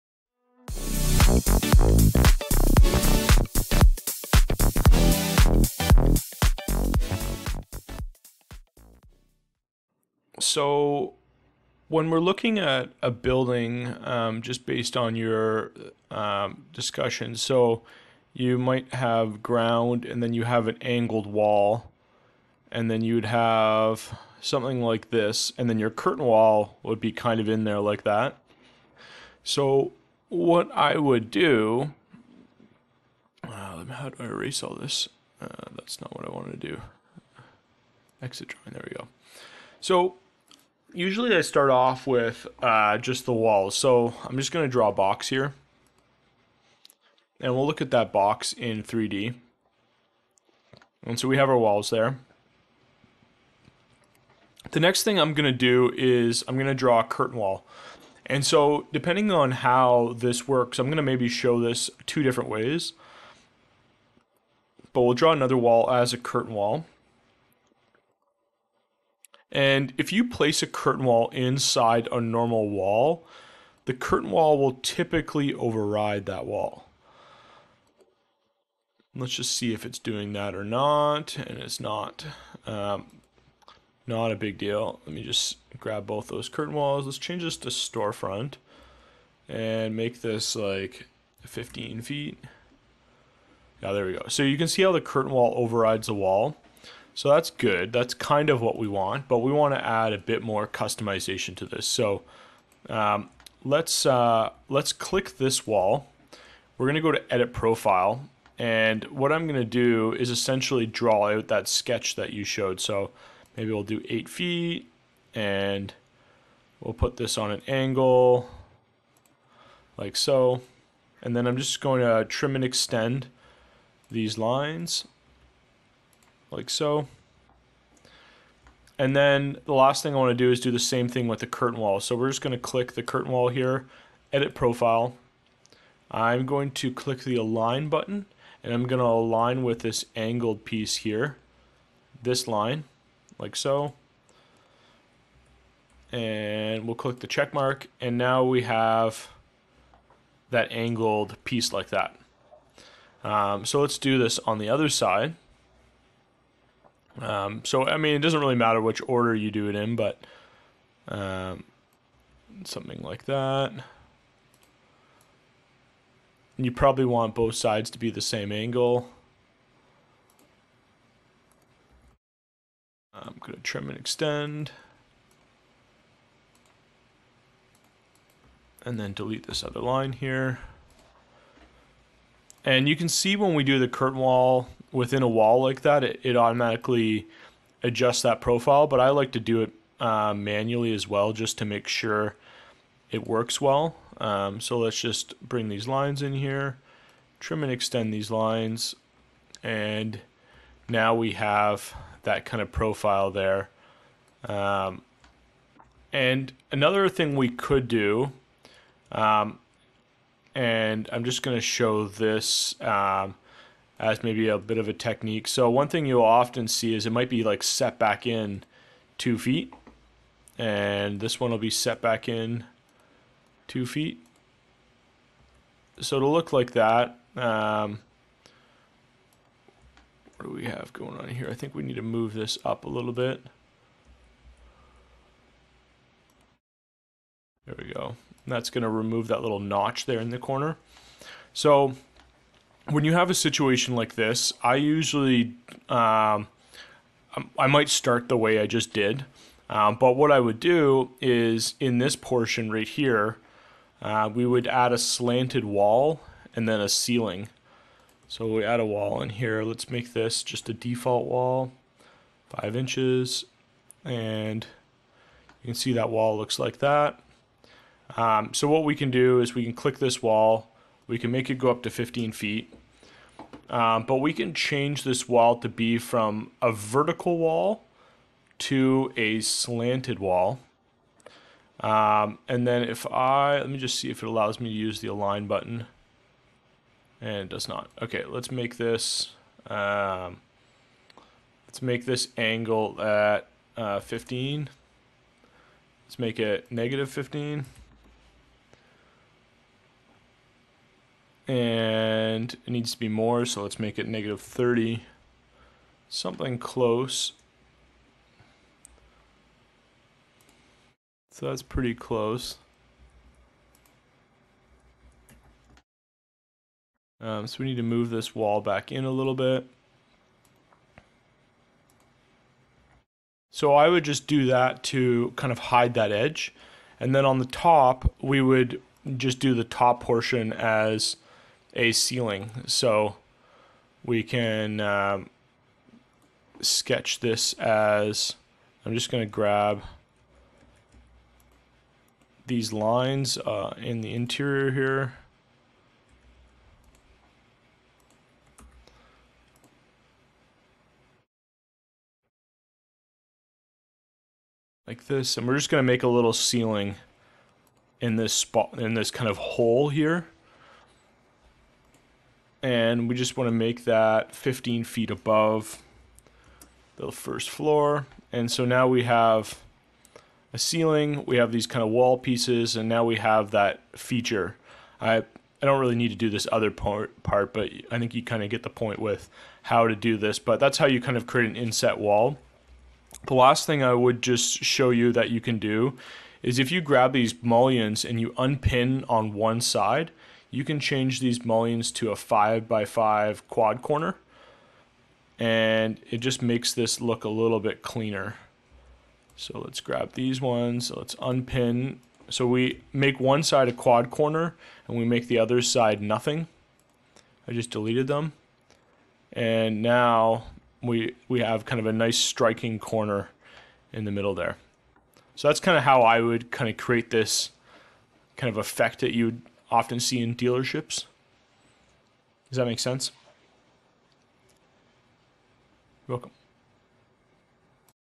Revit Tutorial - How to model sloped or angled Curtain Wall side for facade
Creating Slanted Walls with Curtain Walls in Revit for Showroom Designs
Introduction
In this tutorial, we explore how to create slanted walls with embedded curtain walls using Revit. This technique is often used in showroom or dealership designs where clean, striking architectural features are required. The tutorial covers essential steps like customizing the slant of walls, adjusting curtain wall profiles, and refining mullions for a polished look. Here's how you can recreate this effect in Revit.

1. Drawing the Base Walls and Curtain Wall
The first step in creating the slanted wall design is to set up the base walls and add a curtain wall. Curtain walls are flexible in Revit, allowing you to embed them within normal walls.

Key Steps:
Start by drawing your base walls, creating a simple box or structure.
Insert a curtain wall inside the regular wall, using the storefront type.
Check to ensure that the curtain wall overrides the regular wall, displaying correctly in the 3D view.
2. Adjusting Wall Profiles to Create a Slant
To create the slanted effect, both the regular wall and the curtain wall need to be adjusted. The Edit Profile tool allows you to manually reshape the walls and create the desired angle.

Key Steps:
Select the regular wall and click Edit Profile to modify its shape.
Sketch the slanted wall by adjusting the height and angle, e.g., making one side 8 feet and the other slanted down.
Do the same for the curtain wall, aligning it with the slanted regular wall by using the Align tool to match the angles.
By adjusting the profiles, you achieve the slanted appearance while keeping the curtain wall aligned with the main structure.

3. Insetting the Wall and Adding a Ceiling
To enhance the design, the tutorial introduces the concept of insetting the wall, creating depth and dimension. After creating the slant, you can inset the wall by 2 feet or more, adding further customization.

Key Steps:
Draw an additional inset wall inside the slanted wall to create a recessed section.
Use the Modify Sub-Elements tool to further adjust this inset section, ensuring it fits smoothly with the design.
Add a ceiling to cover the top portion of the slanted wall. Sketch the ceiling within the inset and align it to the height of the wall, ensuring it fits the 15-foot height.
This step adds more complexity and realism to the design, especially for projects where slanted walls are a key architectural feature.

4. Refining the Curtain Wall Mullions
The final touch involves refining the mullions on the curtain wall. The tutorial shows how to replace standard mullions with quad corner mullions to create a clean, striking effect.

Key Steps:
Select the mullions in the curtain wall and unpin them.
Replace one side of the mullion with a 5x5 quad corner mullion, and delete the mullion on the opposite side for a cleaner look.
This technique creates sharp corners, a feature often seen in showroom or dealership designs.
By customizing the mullions, the curtain wall achieves a professional and polished appearance that enhances the overall design.

Conclusion
Creating slanted walls with curtain walls in Revit adds a dynamic and modern element to architectural designs, especially in showrooms and dealerships. By adjusting wall profiles, adding insets, and refining mullions, you can create a visually striking structure that stands out. This tutorial demonstrates how to combine these techniques to achieve a professional and functional result in Revit.